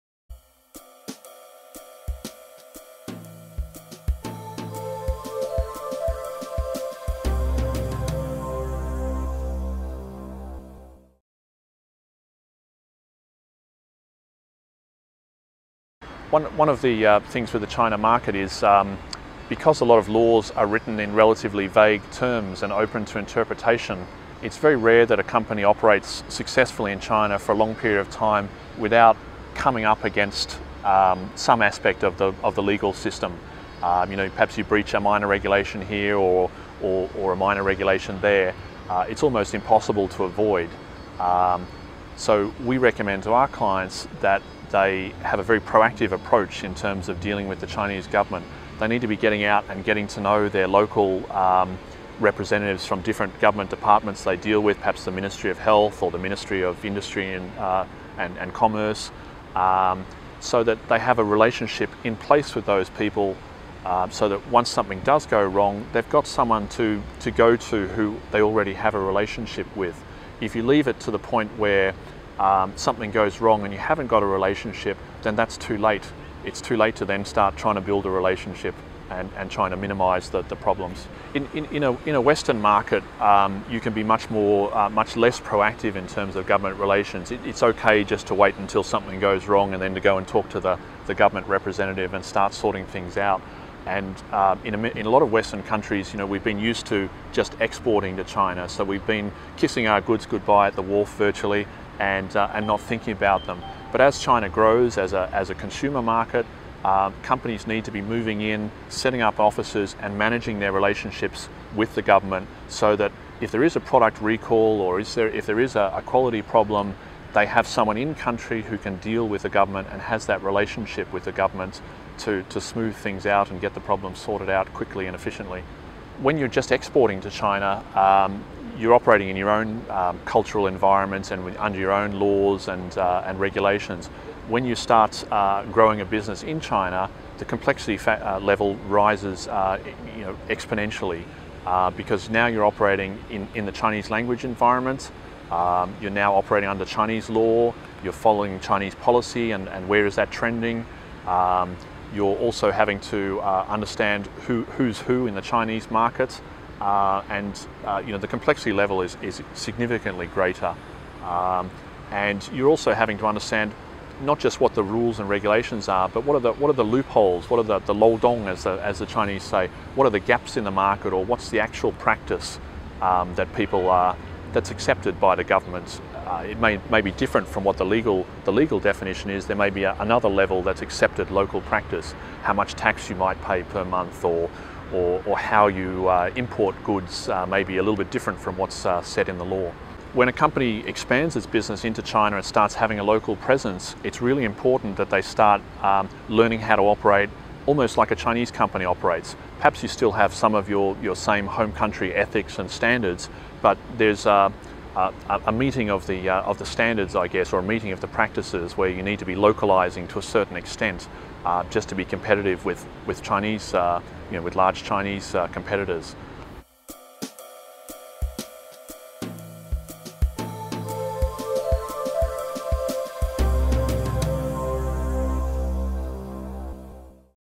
Be proactive with your government relationships - Edward Smith - ClarkMorgan Insights
Business in China requires a closer relationship with government bureaus and departments than is normally expected in the West. This is because of the high number of regulations and laws, which are often ambiguous, and can land inexperienced companies in trouble. Rather than wait until trouble has developed it is wise to establish a relationship earlier.

Edward Smith is the Managing Director of the Beijing Consulting Group (BCG), an Australian owned business consulting firm based in Beijing. BCG assists clients to enter the China market, providing entrepreneurial services ranging from market research, to business registration, to accounting and legal advice, and in-country representation.